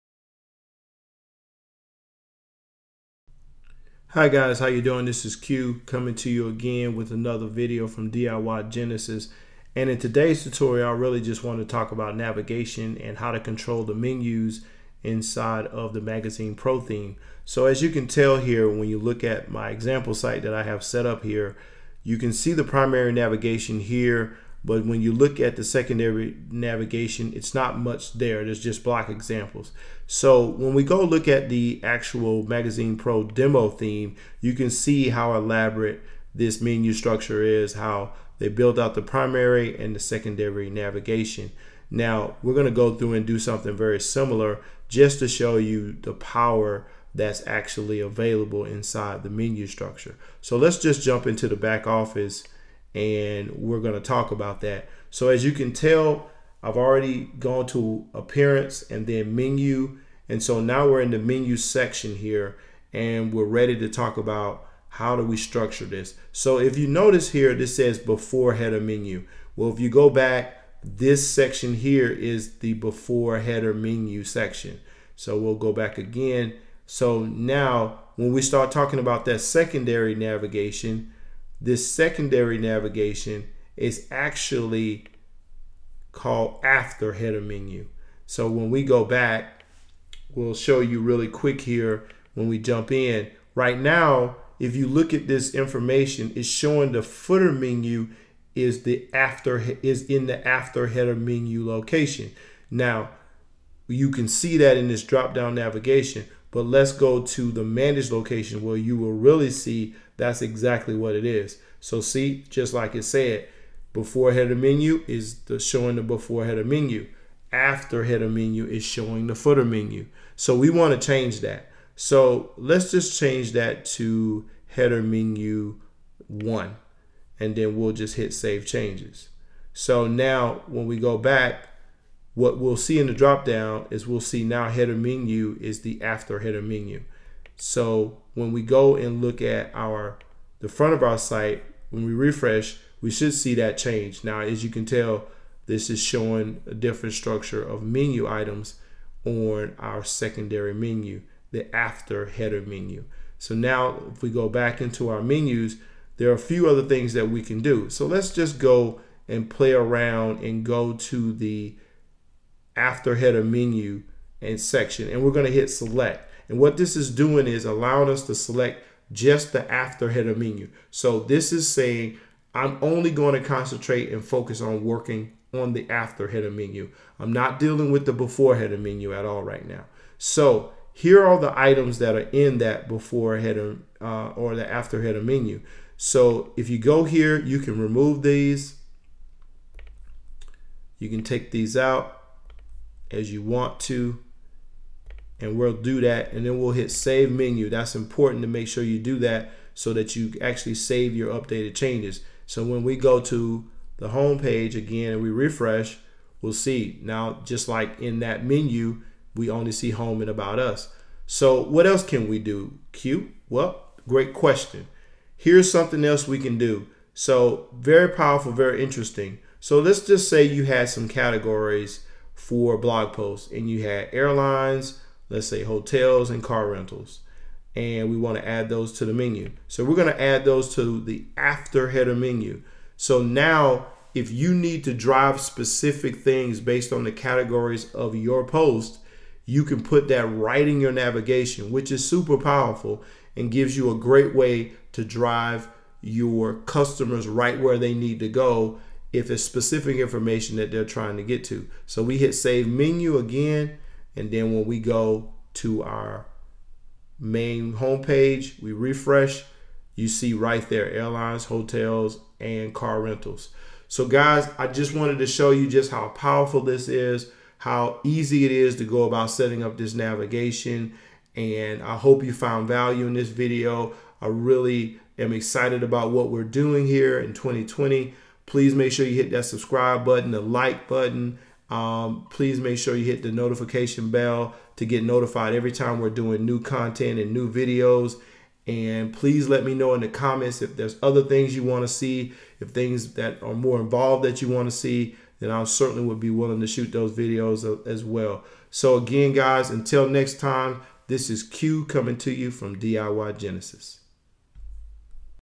Magazine Pro Theme StudioPress 2020 - How To Setup Your Menu Tutorial
Ever wonder how to setup your menu in the Magazine Pro Theme?  Well, in this tutorial we are going to show you exactly how to setup and add categories to your menu.